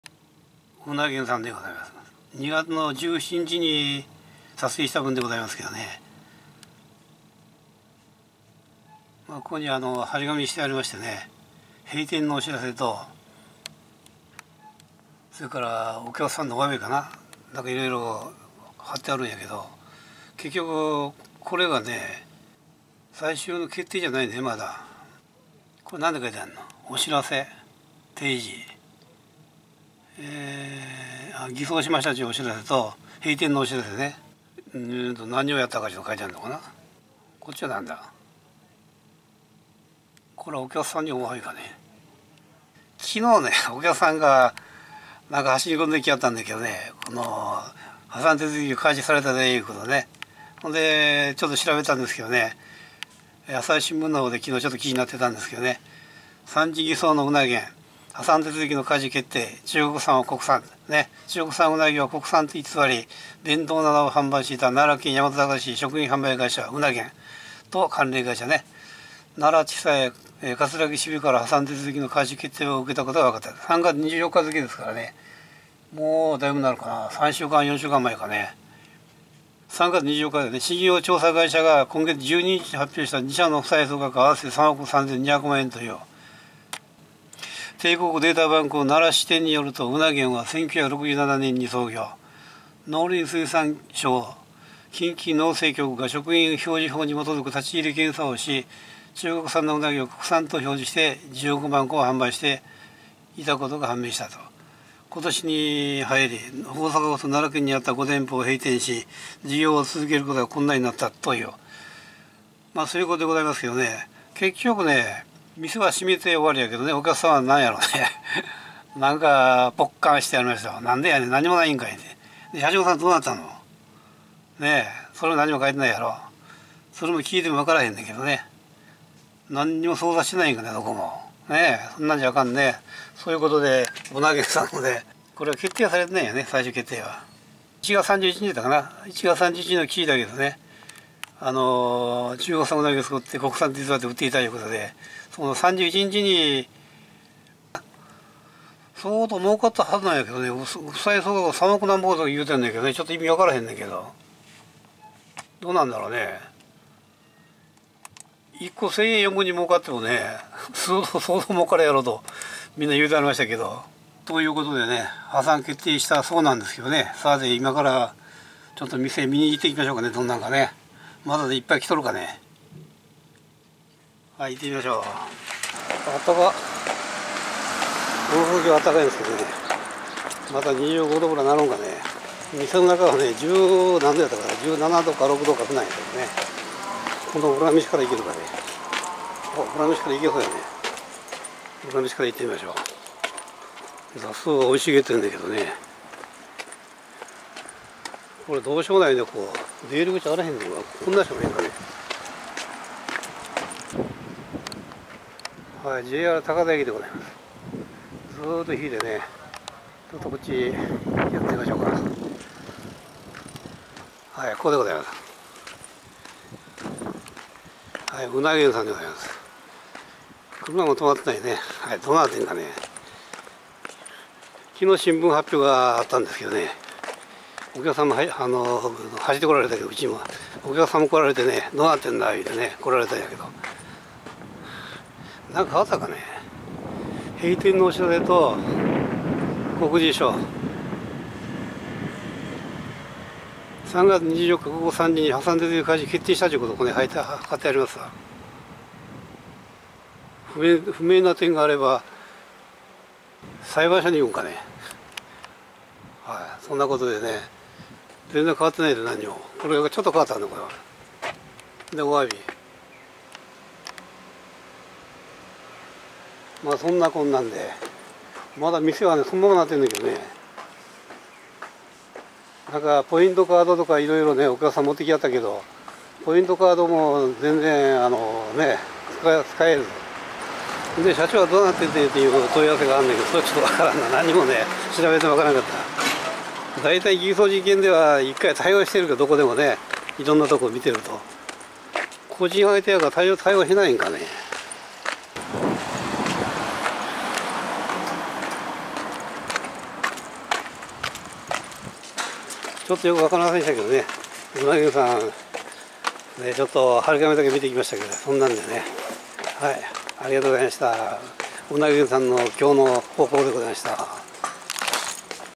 4月13日動画分（UPし忘れて）うなぎ専門店「うな源」昨日聞いたので 前回の動画確認して 店を見に行ってきましたが・・
中国産ウナギを「国産」と偽り、弁当などを販売していた奈良県大和高田市の食品販売会社「うな源」と関連会社「M’S　EEL」が、奈良地裁葛城支部から破産手続きの開始決定を受けたことが分かった。3月24日付。信用調査会社が今月12日に発表した。2社の負債総額は合わせて3億3200万円という。
帝国データバンク奈良支店によると、うな源は1967年に創業。農林水産省近畿農政局が食品表示法に基づく立ち入り検査をし、中国産のウナギを国産と表示して約15万個を販売していたことが判明した。
今年に入り、大阪府と奈良県にあった5店舗を閉店し、事業を続けることが困難になっていたという。